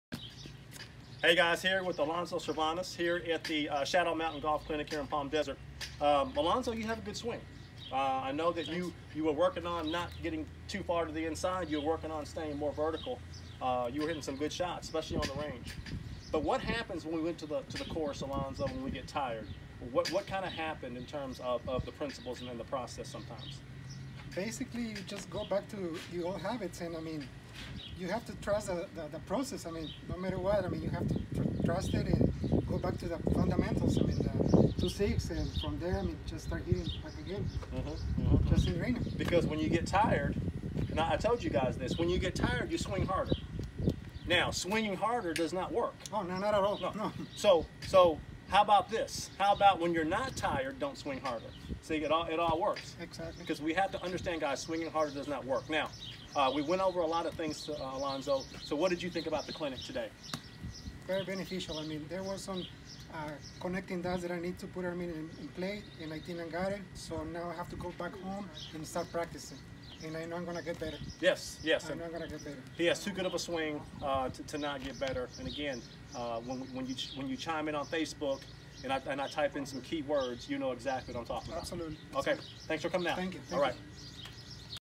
Golf lessons: Palm Desert, CA
Guys in this video listen to Alonzo and his experience with the simple swing 2/6 golf process.